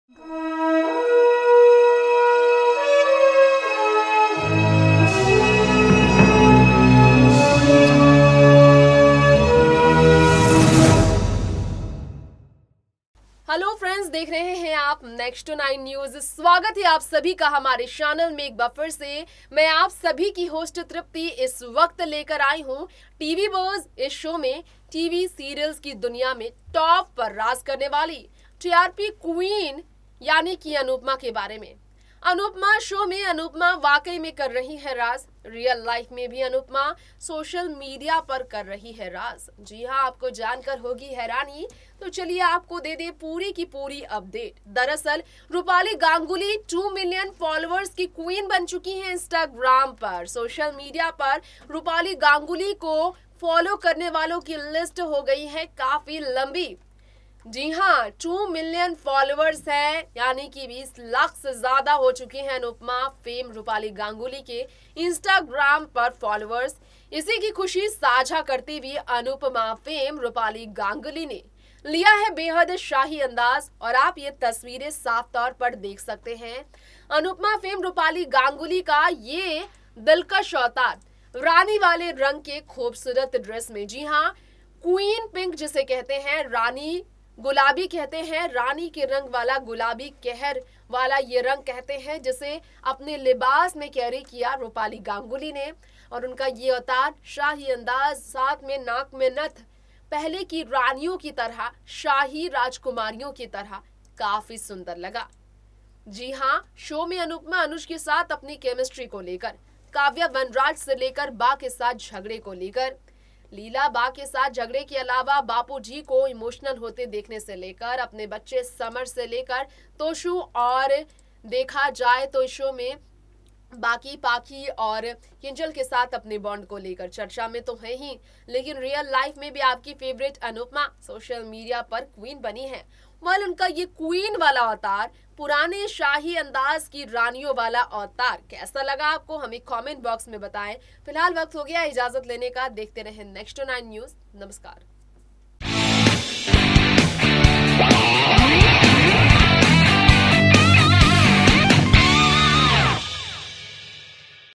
OMG! Anupama Fame Rupali Ganguli का शाही अंदाज, क्या खूब दिख रही हैं अनुपमा...!
सीरियल अनुपमा में अब तक अनुज और अनुपमा की कहानी बेहद शानदार चल रही हैं लेकिन बात यहां हो रही हैं अगर अनुपमा फेम रुपाली गांगुली के खास अंदाज़ की तो आपको बता दे आपकी फेवरेट रुपाली गांगुली जो हैं...2 मीलियन फोलोअर्स होने की वजह से शाही अंदाज में फैंस का शुक्रियादा करती नजर आई हैं